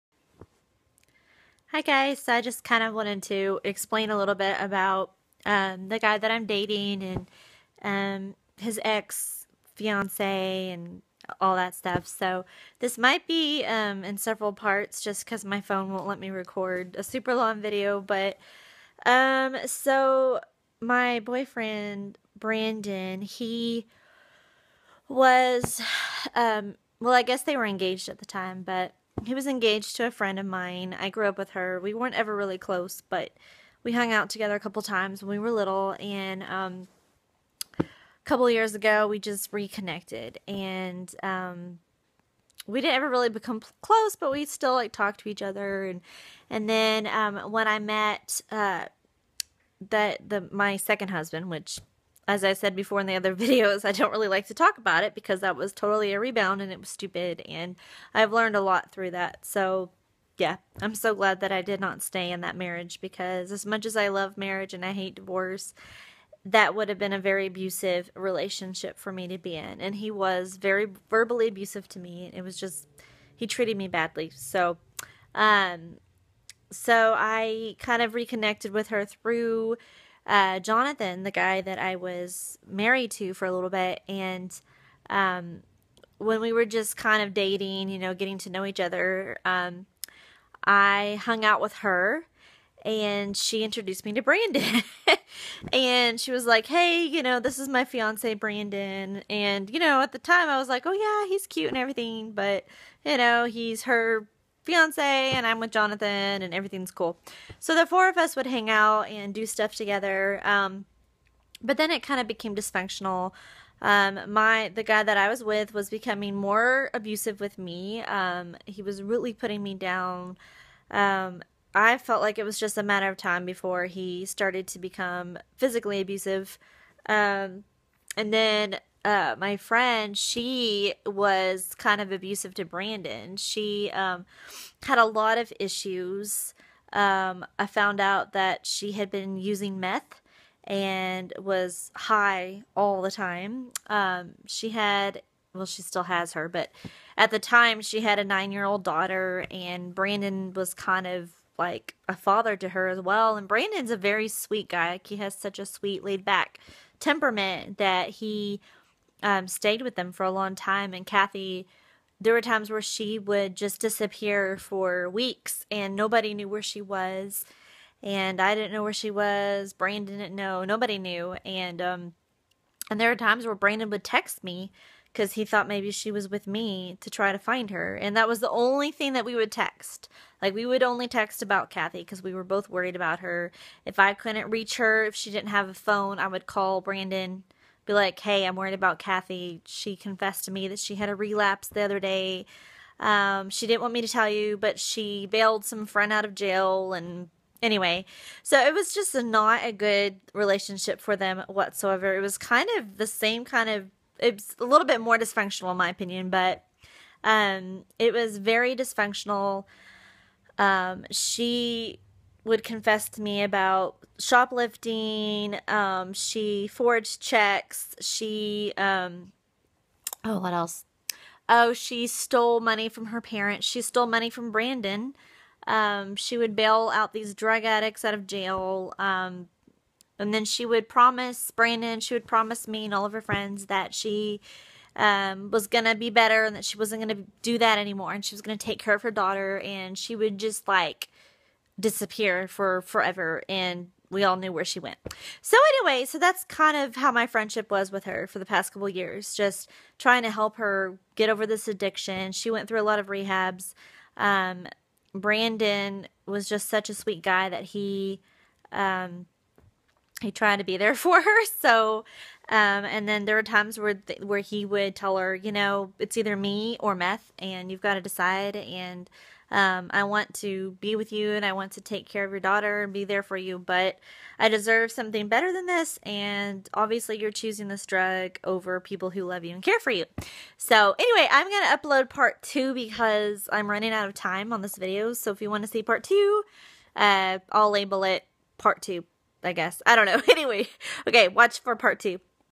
My boyfriends ex and how she feels
How does my ex friend feel about me dating her ex boyfriend?